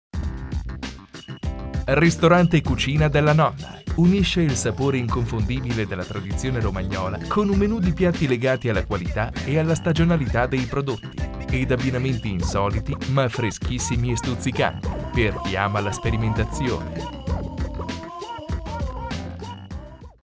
CUCINA DELLA NONNA RIMINI (RIMINI)
CUCINA DELLA NONNA 77, V. S. AQUILINA Tel Unisce il sapore inconfondibile della tradizione romagnola, con un men? piatti legati alla qualit? alla stagionalit?ei prodotti, ad abbinamenti insoliti ma freschissimi e stuzzicanti. Per chi ama la sperimentazione